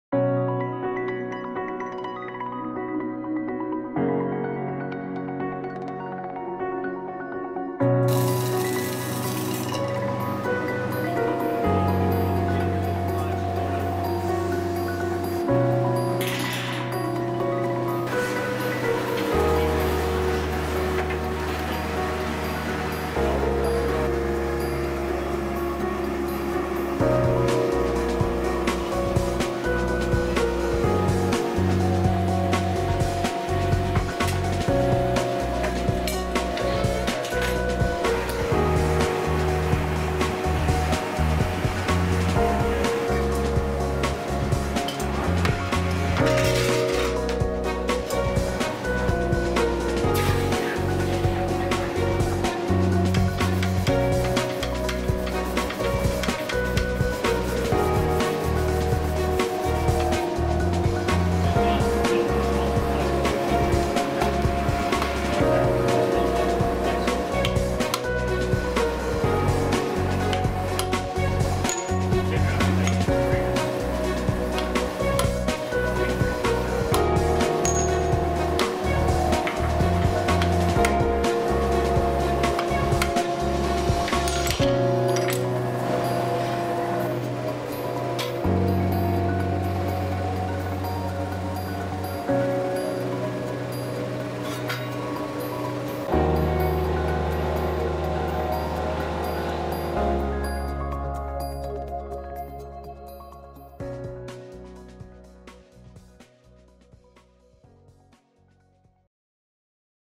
Buy Silver Bullion Refining and Casting - HD
Pure silver grain being melted and poured into cast silver bullion for investment. See the amazing process of crafting the highest quality bullion made in New Zealand